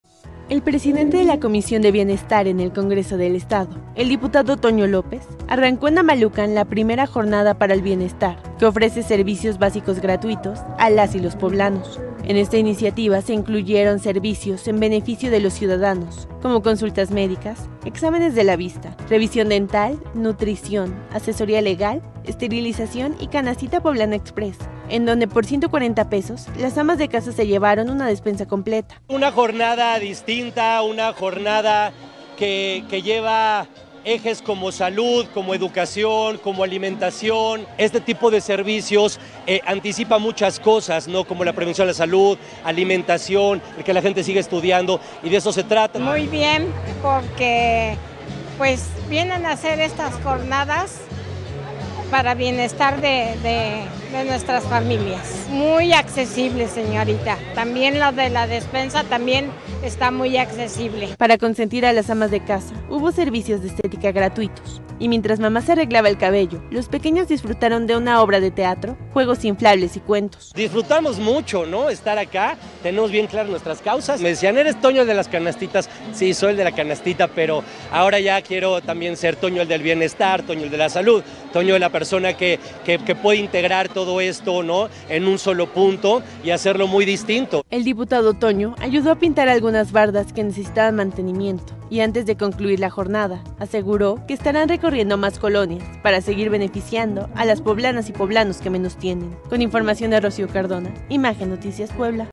Jornada Bienestar Toño López || Noticias con Juan Carlos Valerio
Diputado @soytonolopez lleva a cabo jornada de Bienestar que incluye apoyos en salud, educación, economía y más.

Mi compañera @RocioCardona712 nos presenta los detalles.